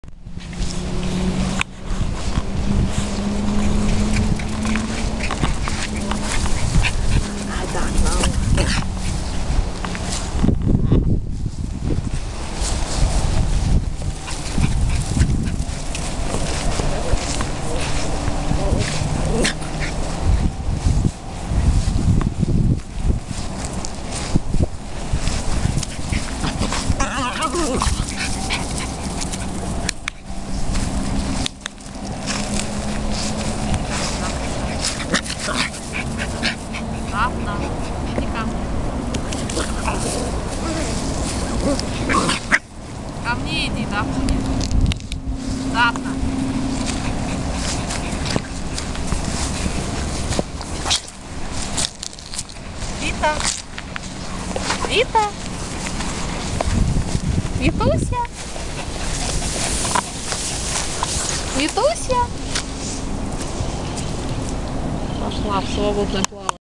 Испания, ноябрь 2013 г. Бишоны "DaVita's Heart".
Отпуск в Испании.
Наши бишоны на прогулке.
Дафна (Kuner's Kiss by Kiss), 19 мес. + Вита (Camellia bloom Vitalina), 9 мес.